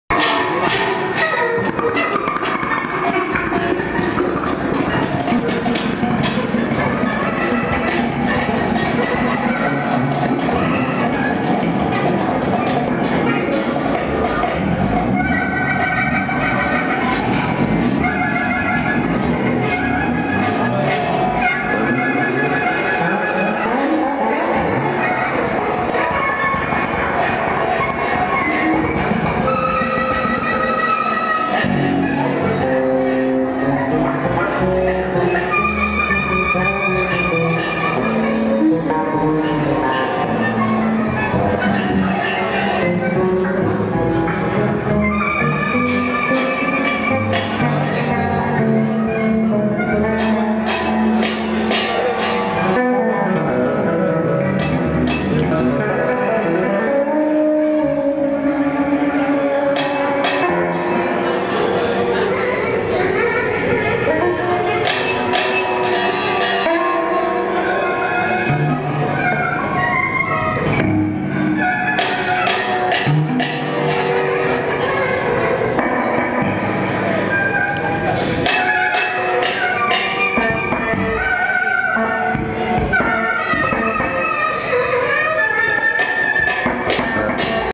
Derek Bailey Tribute Band - Jazz Café San Pedro Mayo 24 2012 - Part 2
Gabriel Wiernik (Bit-0) y Alex Catona (Cato) - Jazz Café San Pedro Mayo 24 2012

Este 24 de mayo JAZZ CAFE SAN PEDRO y 29 de mayo en EL STEINVORTH Gabriel Wiernik (Bit-0) y Alex Catona (Cato), ambos, experimentados musicos e improvisadores de reconocida trayectoria internacional, rendirán homenaje a uno de sus heroes y mayores fuente de inspiración: Derek Bailey.

Derek Bailey, (29 Enero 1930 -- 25 Diciembre 2005) fue un guitarista ingles cuyo nombre es omónimo con la vanguardia mundial de la improvisación libre. Nativo de Sheffield, Derek Bailey empezó a los 10 años tocando guitarra de una manera tradicional y eventualmente, a través de sus colaboraciónes con Tony Oxley, Gavin Bryars, Fred Frith, Anthony Braxton, John Zorn, Evan Parker, Steve Lacy, Pat Metheny, Cecil Taylor, etc...desarrollo su propio estílo, tan personal como particular.